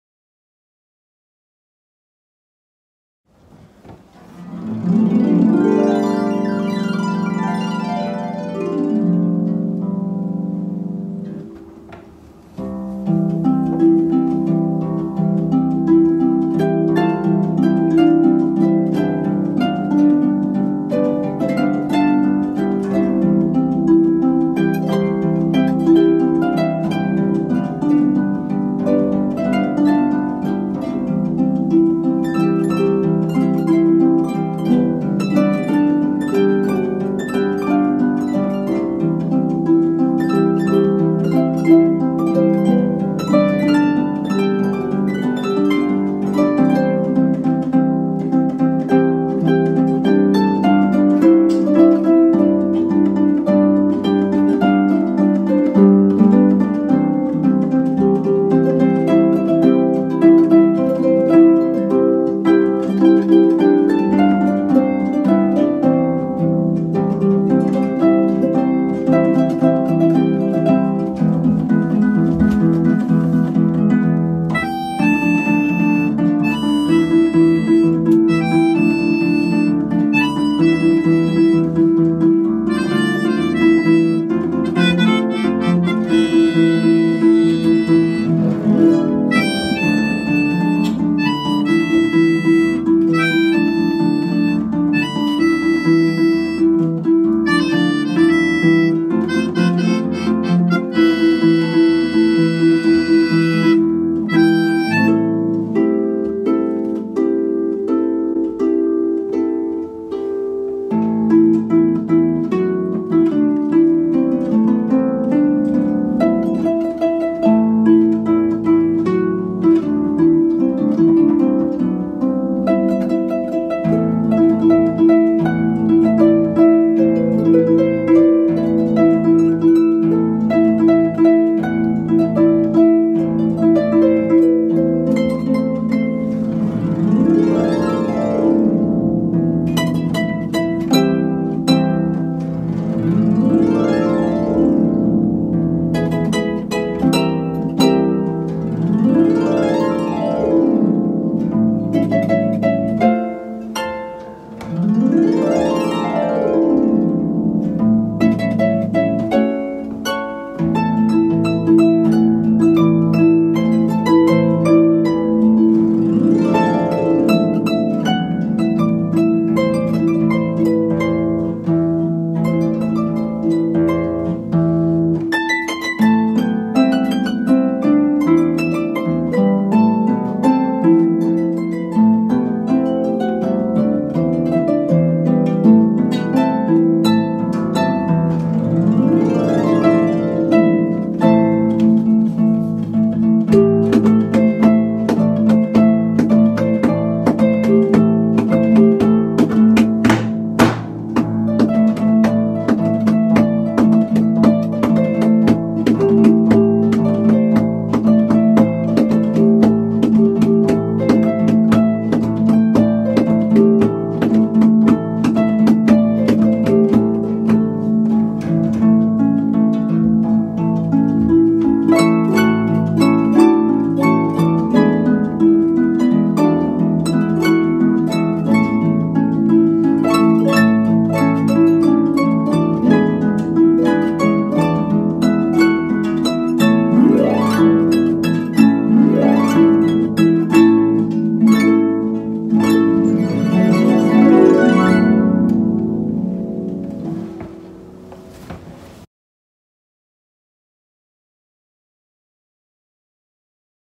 Festival medley on 2 harps 祭りメドレー
Festival medley on 2 harps

arrannged by Kyoko Okuda
Performed by Les Colocs

祭りメドレーMEDLEY on 2 harps

編曲　奥田恭子

演奏　ハープデュオ　「レ・コロック」奥田恭子
　　　新井コルチ薫